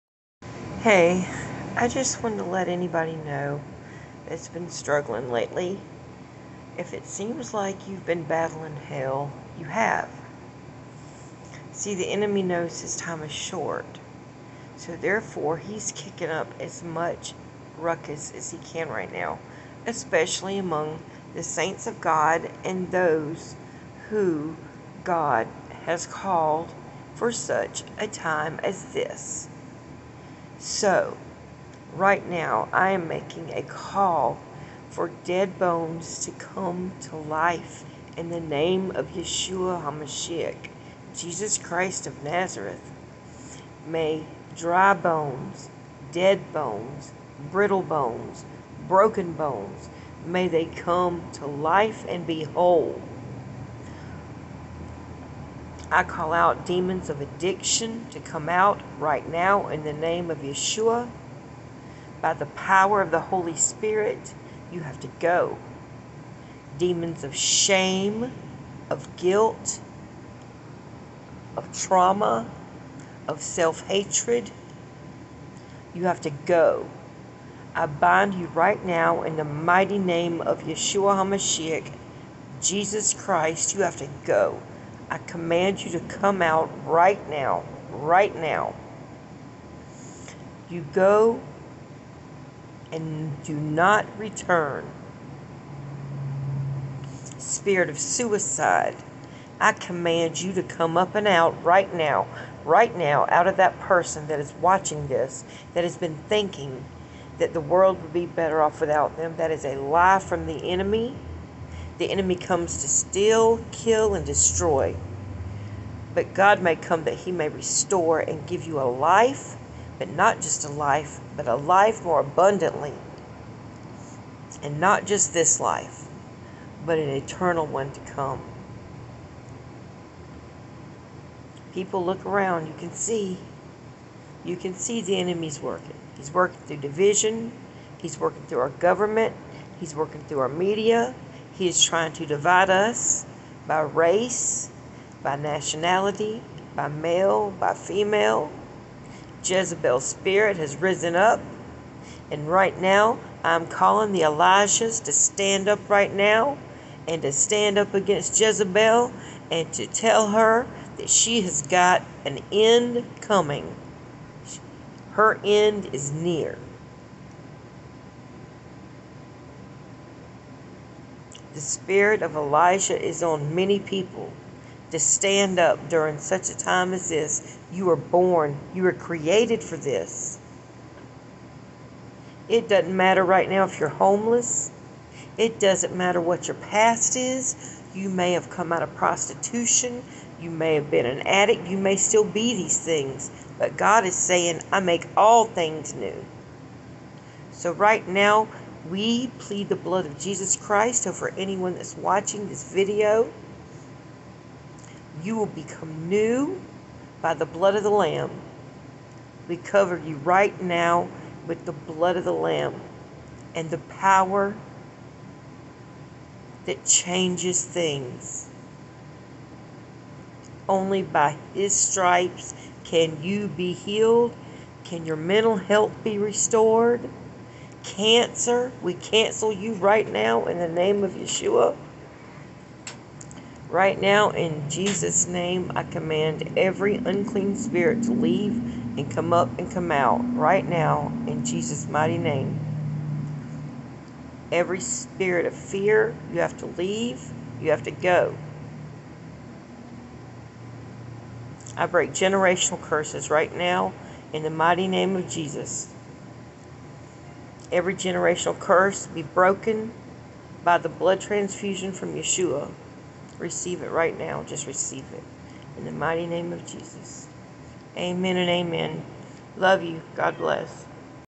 Struggling ? Let the blood of Yeshua set you FREE! Whom the son sets free is FREE indeed!
Break free and fulfill you're destiny, the plan that God the most high YHVH has for you, be filled with the holy spirit and receive deliverance, and healing and salvation through the blood of the lamb, and is coming back as the LION OF JUDAH🙌✝️💜